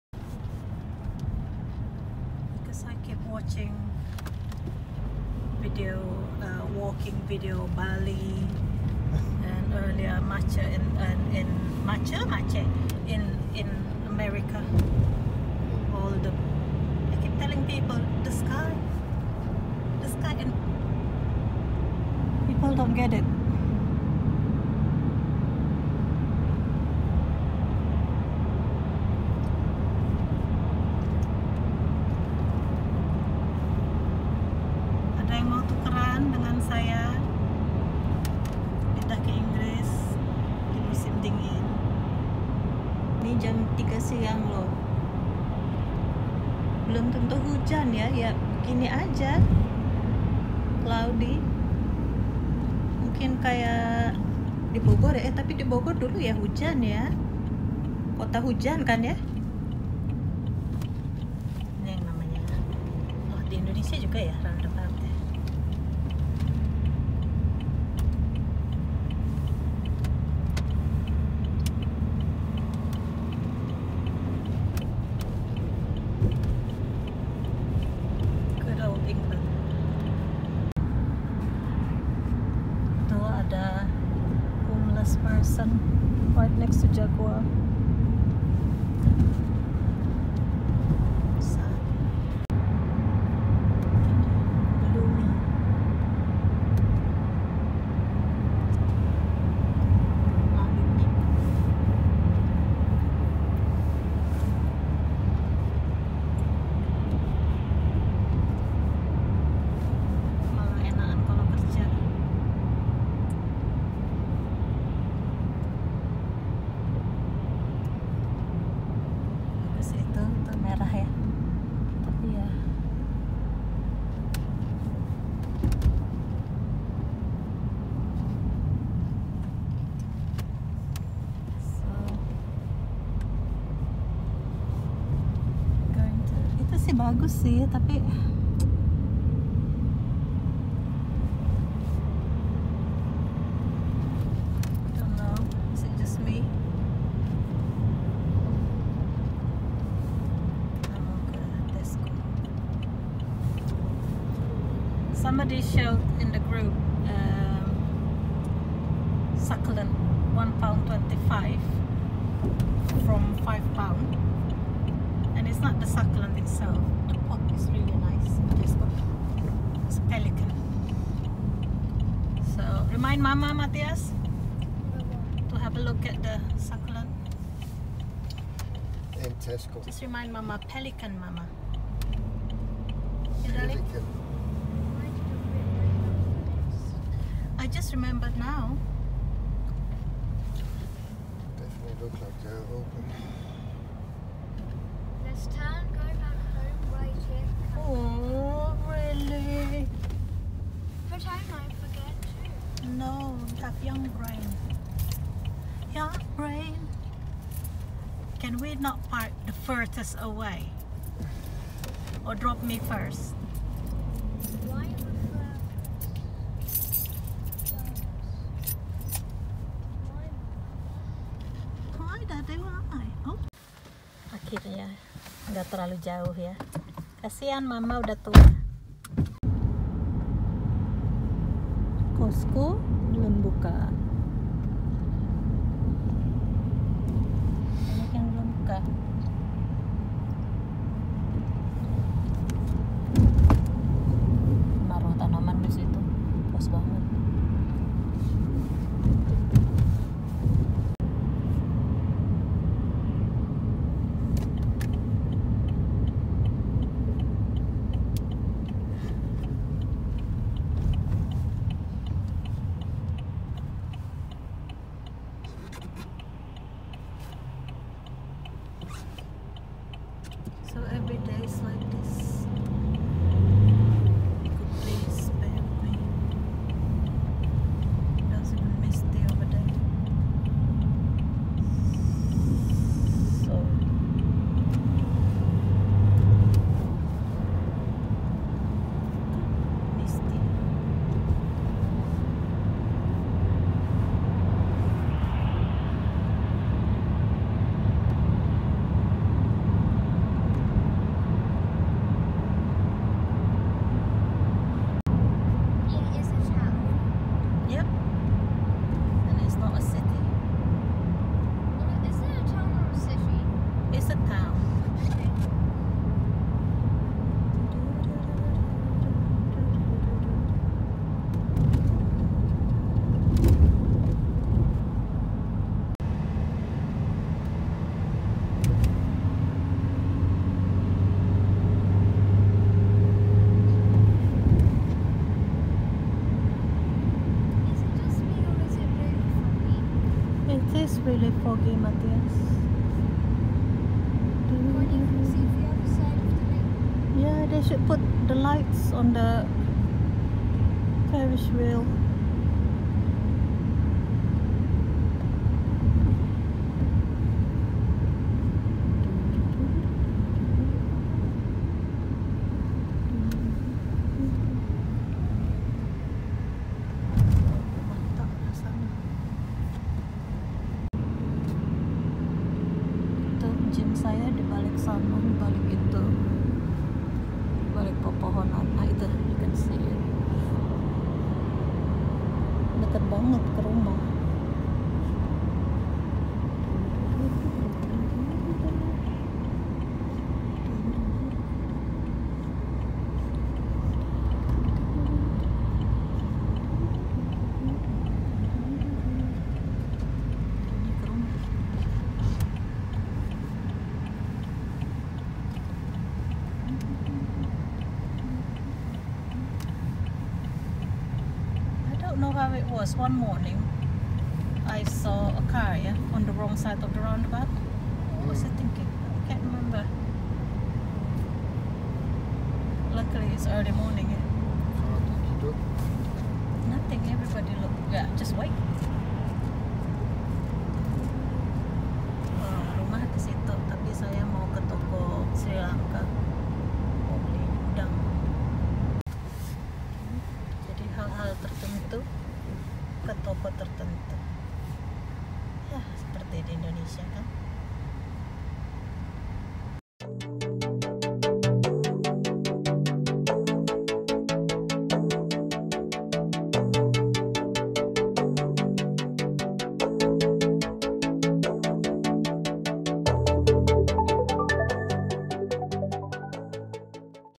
Winter Sky of England - Langit Musim Dingin di Inggris
This might explain why I love watching walking video from Bali, US, Korea, anywhere with big blue sky. Serves as mood booster for me :)
Semoga memberi gambaran kenapa saat ini saya sering nonton video orang dari Bali, US, Korea, pokoknya yg langitnya cerah deh. Bagus utk mood saya, di saat langit kami lebih sering kelabu.